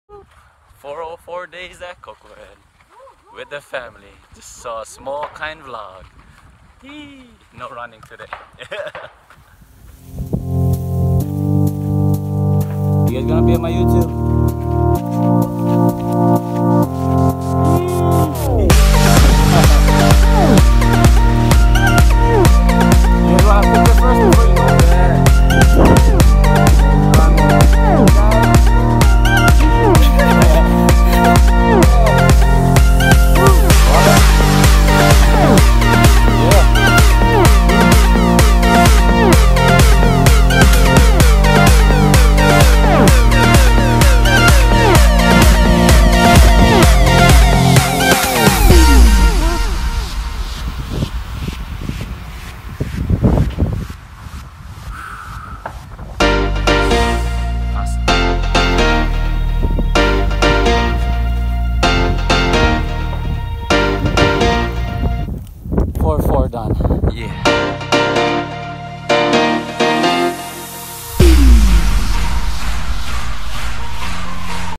Koko Head Hike Vlog! 404 Days! (50min. 20sec.)
Today is not my usual run, but just a quick vlog. I wanted to make sure everyone was okay. I still got some workout though and learn a few things about the trail to help me go faster.

Koko Head trail hike is also known as Koko Head Stairs, Koko Crater Stairs, or just Koko Head. I run up here everyday, but before I explain why, here are some tips for this hike.

 Please bring hiking attire, water, camera, and a walking stick if needed. Respect the trail and don't liter, be mindful of others, don't throw your shoes up on the telephone lines, and if you ever feel dizzy, STOP and TAKE A BREAK! Catch your breath, drink water and keep going up if you can, but don't force yourself! You can hike Koko Head another day. Trust me, I have been hiking here since 2007 and I have seen people fainted and airlifted. It doesn't look pretty.

 This is my 404 Days running up Koko Head! Today I hiked it in fifty minutes and twenty seconds with my GoPro Hero 5 Session!

  Now for the reason why I run up here is that, I want to be the fastest to go up Koko Head since 2012. I only got serious about achieving this goal since last year June 6, 2017. With a lot of inspiration from family, friends, and with the right timing I decided to do this every day so I can get my body used to the trail.

 A disclaimer, what I mean by everyday, is that I go up and down Koko Head 2 or 3 times in that same day so I can make up for the next couple/few days ahead due to other things I have going in my life. I start from my car and hike all the way up to the helicopter pad to make it a full day. Then I go back down to my car. Drink water or eat a snack if I needed it and go back up and down again.

 I start the Runkeeper app at the beginning of the railroad ties and stop it at the top of the helicopter pad. The fastest time according to the Strava app was 9:07 running this same distance! That is the time to beat! My fastest so far was 11:59. I have 3 minutes to cut down in order for me to get my personal record! I believe I can do it with the help of God. He has been with me through each step of the way, I couldn’t get through all of this without His help. God has given me the strength and wisdom to endure this challenge through a lot of praying. If you guys need help with anything, you guys can talk to God and he will listen to you.

 Here we go! Let’s get fit and enjoy the video! Have a Blessed Day!

WOOP WOOP!

You can also find me here as well.